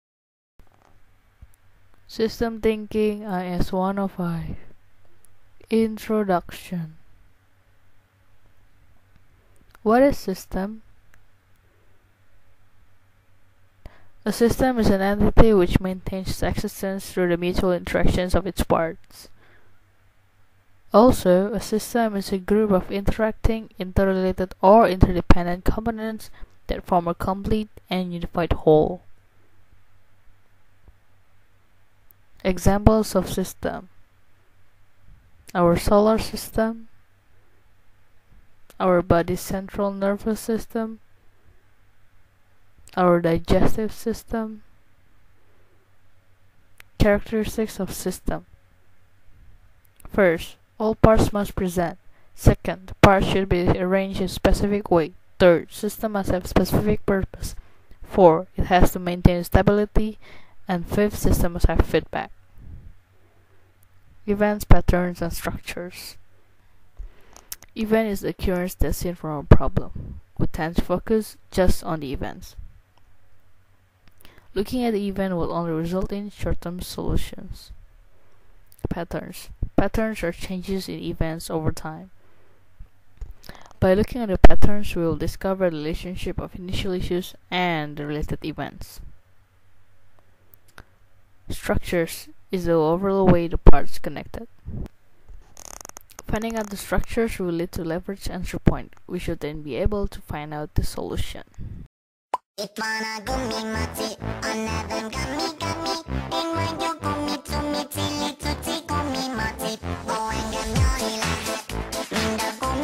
System Thinking Introduction
This is my first time in making video. It's about system thinking. The video regards to my first assignment in IS 105 System Thinking class at BiNus Intenational University,

Materials for this video was from the book: Systems Thinking Basics: From Concepts to Causal Loop. Published by Pegasus Communication Inc., 1997 Authors: Anderson, Virginia and Johnson, Lauren.

I'm sorry if this is not a perfect video and lacks in a lot of aspects but hope you'll get something from seeing it. Thank you